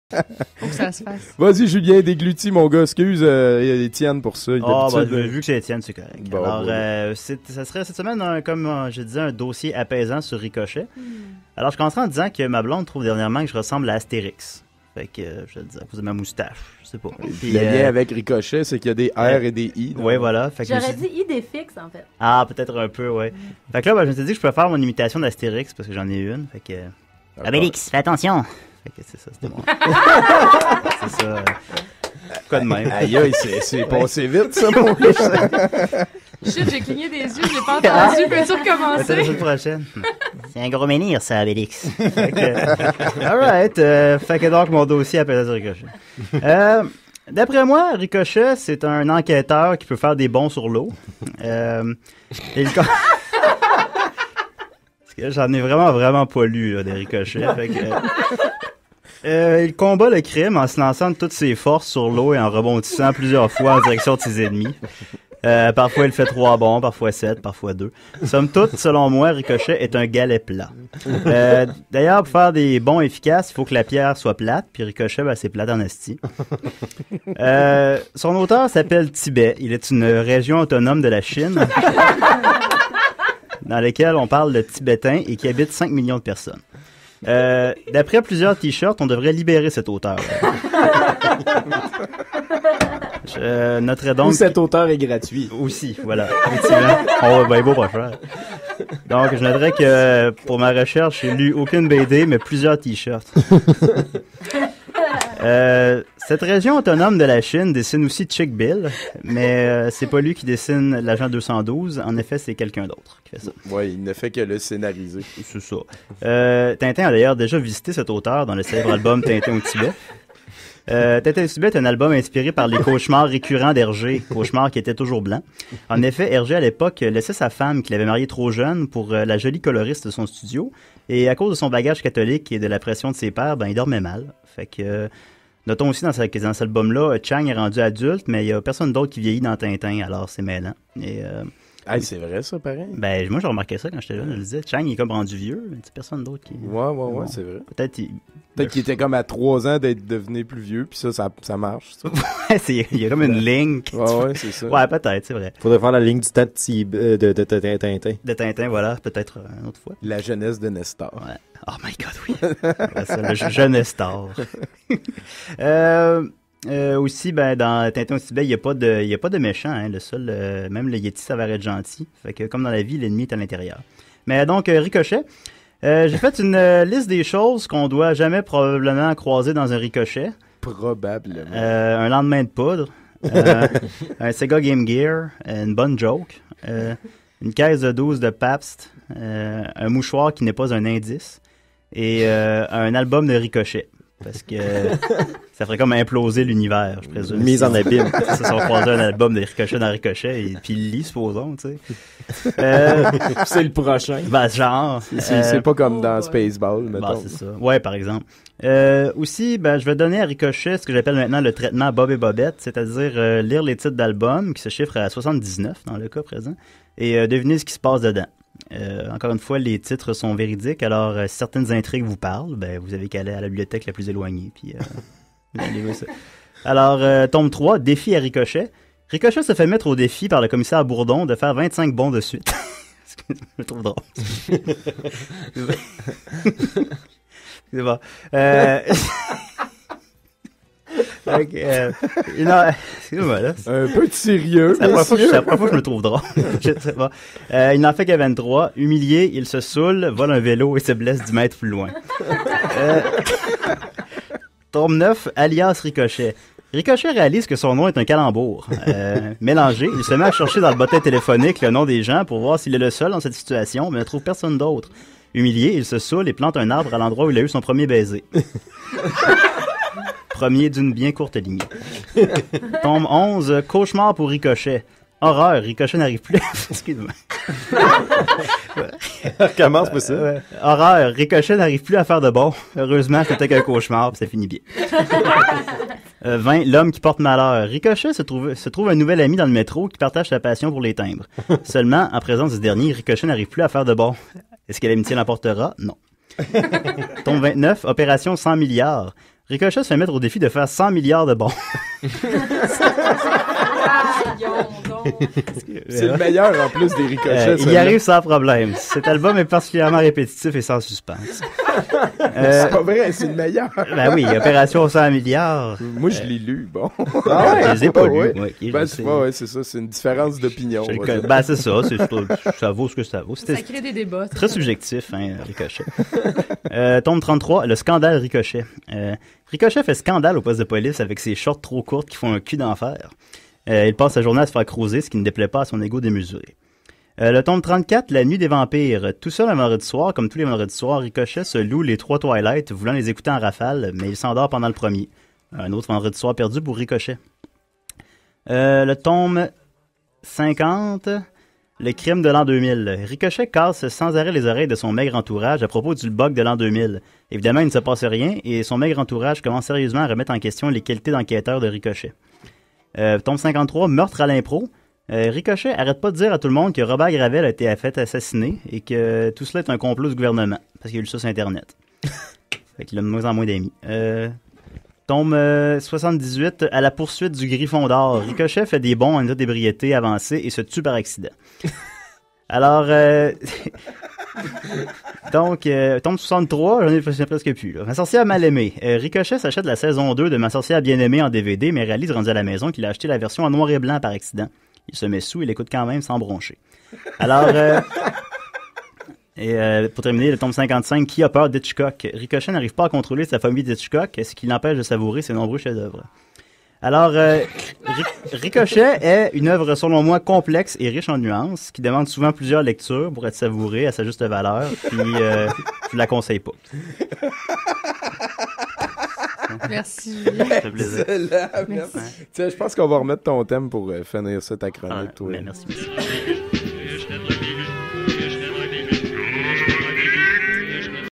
Chronique BD #8 - Ric Hochet
19 juillet 2010

J'étais particulièrement confus cette fois! Je confondais l'auteur Tibet et le pays le Tibet. Et j'étais convaincu que Ric Hochet pouvait faire des bons sur l'eau!